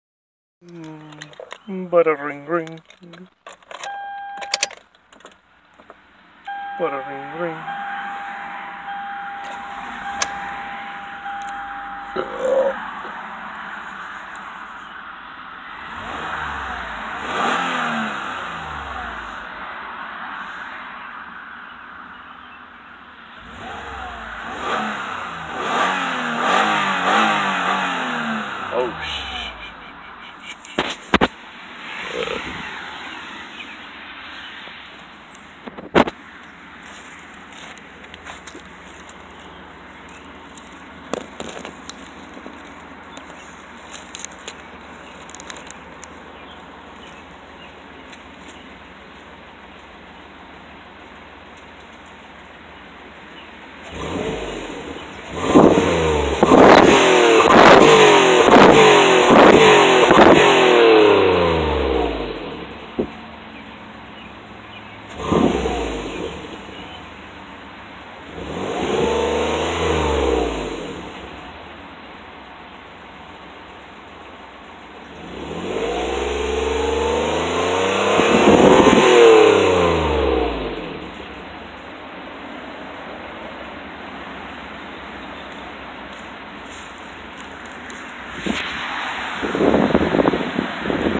V6 Highlander Rev Up
Just messing around, pre- oil change. Castrol 5w-30 conventional.K&N box filter.
v6. DOHC 3.5 Toyota No exhaust mods -0